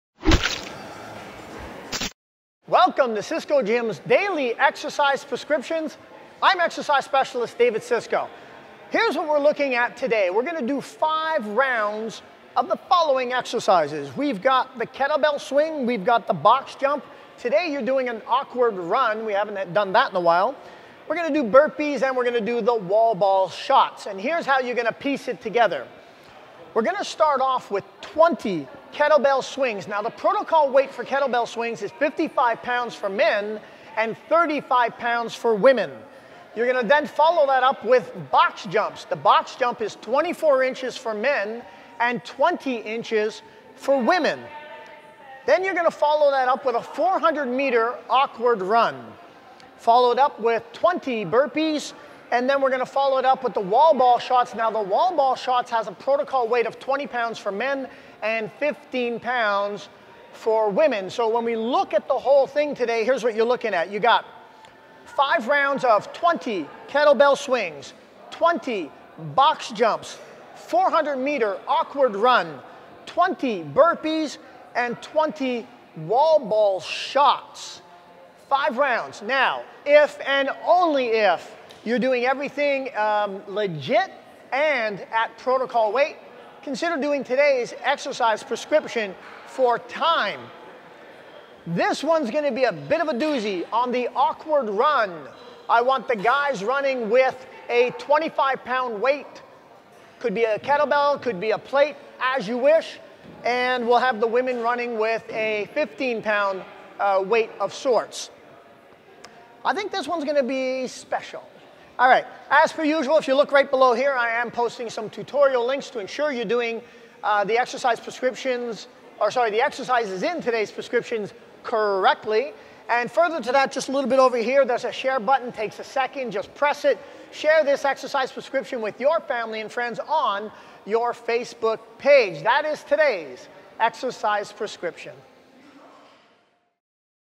Daily Exercise Prescription 12.6.12 Siscoe Gym
Siscoe Gym's Daily Exercise Prescription for 12.6.12

Rx
5 Rounds of
20 x Kettlebell Swing M= 55Lbs / F= 35Lbs
20 x Box Jump M= 24Lbs / F= 20Lbs
400m Awkward Run
20 x Burpees
20 x Wall Ball Shots M= 20Lbs / F= 15Lbs